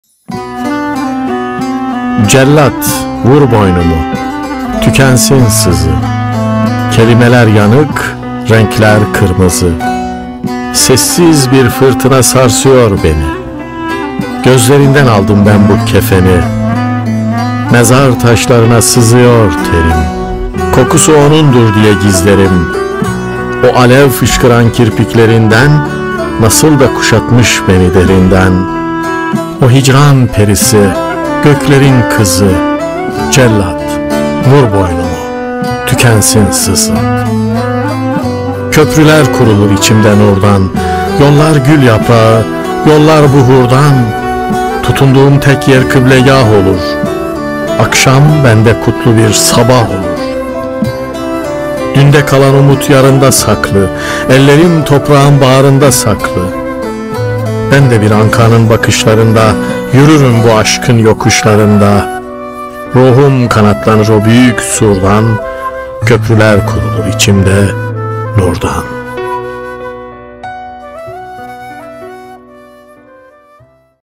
Cellât, vur boynumu, tükensin sızı…| Sızı Şiiri - Nurullah Genç (Kendi sesinden)
Cellât, vur boynumu, tükensin sızı
Kelimeler yanık, renkler kırmızı
Sessiz bir fırtına sarsıyor beni
Gözlerinden aldım ben bu kefeni
Mezar taşlarına sızıyor terim
Kokusu onundur diye gizlerim
O alev fışkıran kirpiklerinden
Nasıl da kuşatmış beni derinden
O hicran perisi, göklerin kızı
Köprüler kurulur içimde nurdan
Yollar Gül yaprağı yollar buhurdan
Tutunduğum tek yer kıblegâh olur
Akşam bende kutlu bir sabah olur
Dünde kalan umut yarında saklı
Ellerim toprağın bağrında saklı
Ben de bir Anka’nın bakışlarında
Yürürüm bu aşkın yokuşlarında
Ruhum kanatlanır o büyük surdan